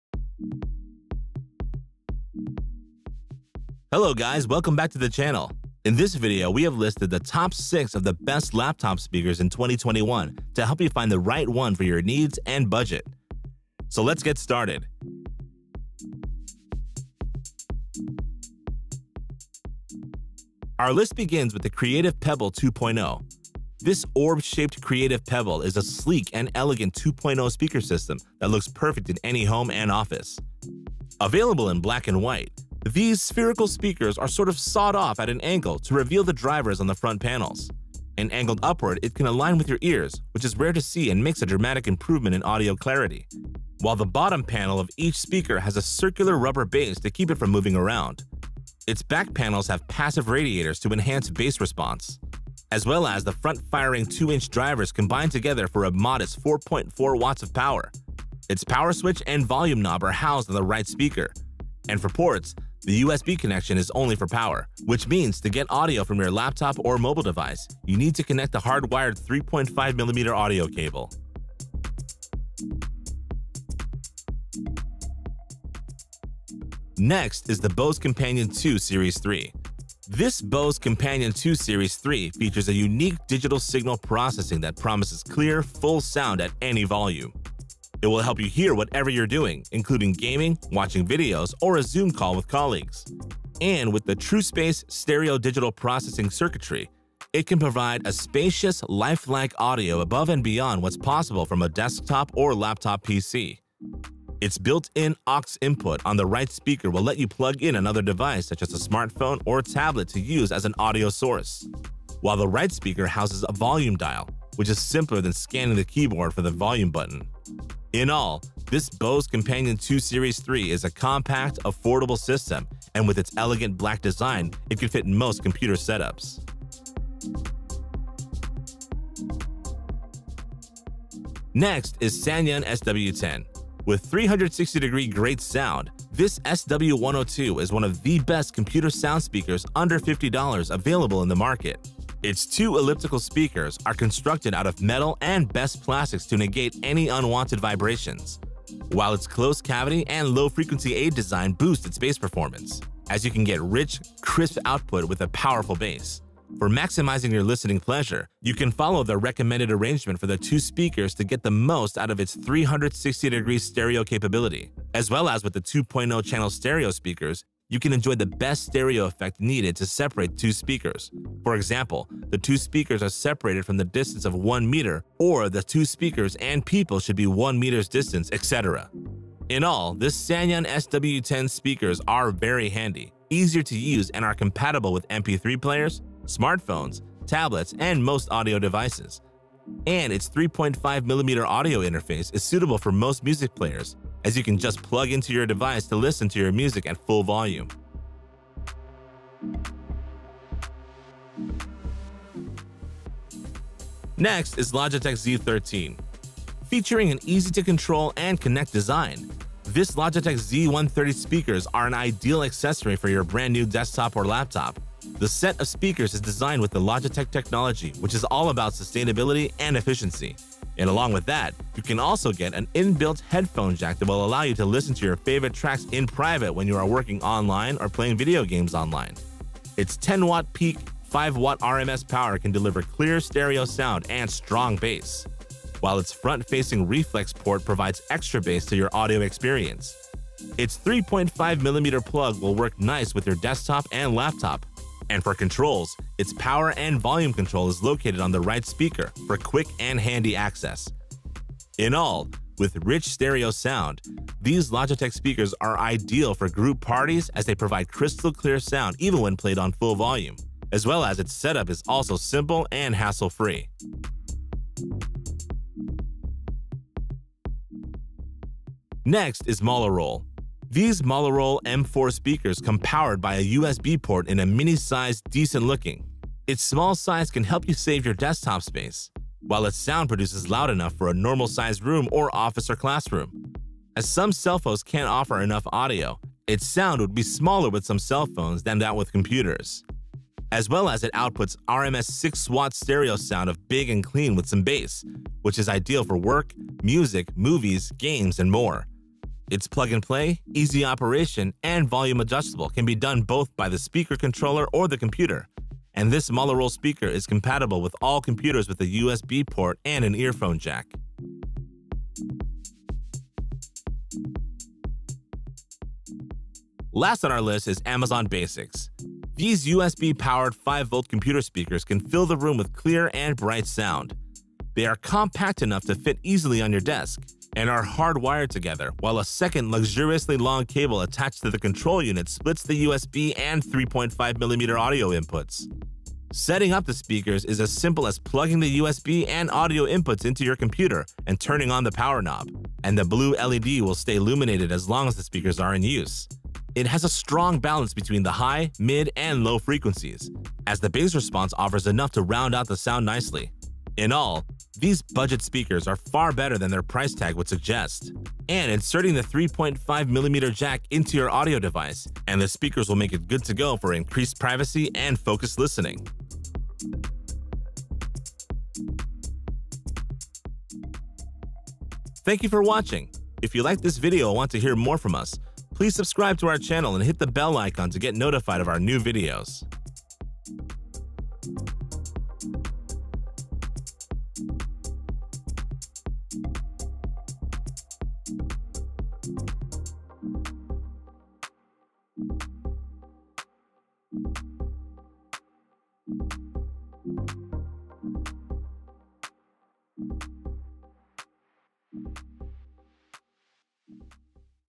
TOP 6: Best Laptop Speakers in 2021 - Which Is the Best for You?
The Best Laptop Speakers In 2021

• Creative Pebble 2.0

• Bose Companion 2 Series III

• Sanyun SW10

• Logitech Z13

• Amazon Basics

TIMESTAMPS
00:00 Intro
00:20 Creative Pebble 2.0
01:16 Bose Companion 2 Series III
02:10 Sanyun SW10
03:23 Logitech Z13
04:28 Moloroll
05:16 Amazon Basics

Our list begins with the Creative Pebble 2.0. This orb-shaped Creative Pebble is a sleek and elegant 2.0 speaker system that looks perfect in any home and office. Available in black and white, these  spherical speakers are sort of sawed off at an angle to reveal the drivers on the front panels. And angled upward, it can align with your ears, which is rare to see and makes a dramatic improvement in audio clarity. While the bottom panel of each speaker has a circular rubber base to keep it from moving around. Its back panels have passive radiators to enhance bass response. As well as the front-firing 2-inch drivers combine together for a modest 4.4 watts of power. Its power switch and volume knob are housed on the right speaker. And for ports, the USB connection is only for power. Which means, to get audio from your laptop or mobile device, you need to connect the hardwired 3.5mm audio cable.
Next is the Bose Companion 2 Series III. This Bose companion 2 series III features an unique digital signal processing that promises clear, full sound at any volume. It will help you hear whatever you're doing, including gaming, watching videos, or a Zoom call with colleagues. And with the Truespace stereo digital processing circuitry, it can provide a spacious, lifelike audio above and beyond what's possible from a desktop or laptop PC. Its built-in aux input on the right speaker will let you plug in another device like a smartphone or tablet  to use as an audio source. While the right speaker houses a volume dial, which is simpler than scanning the keyboard for the volume buttons. In all, this Bose Companion 2 Series III is a compact, affordable system, and with its elegant black design it can fit most computer setups.